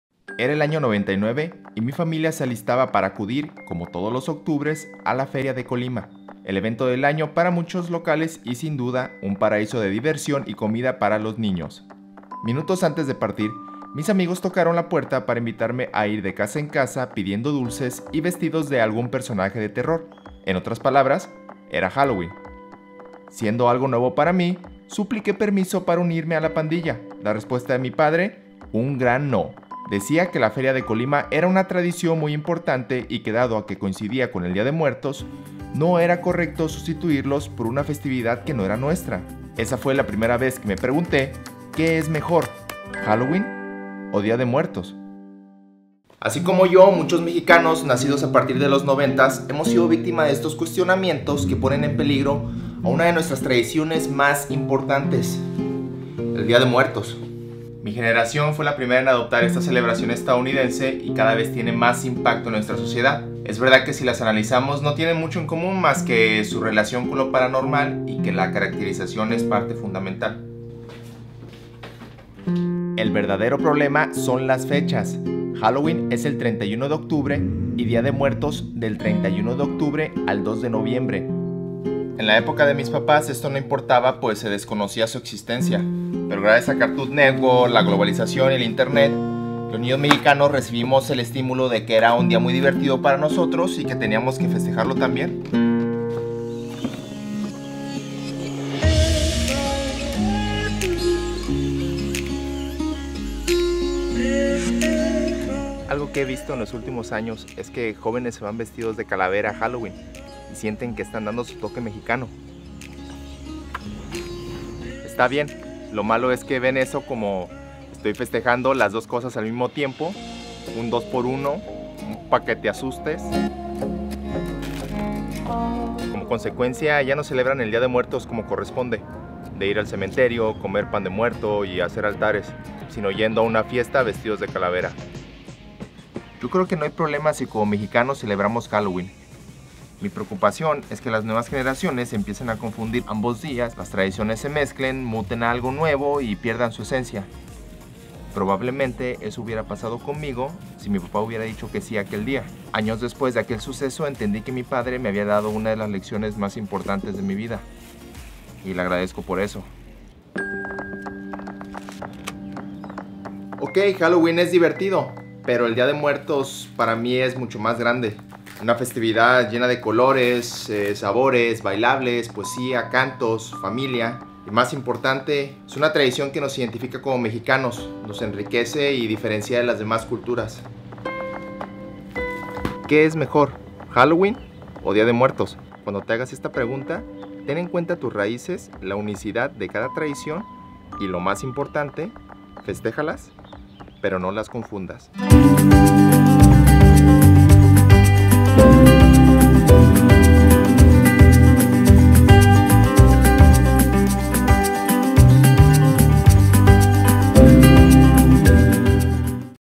Halloween vs Día de Muertos
Halloween es un día que cada vez agarra más fuerza en México. El problema es que coincide con una de nuestras tradiciones más importantes: El Día de Muertos. ¿Tú qué festejas?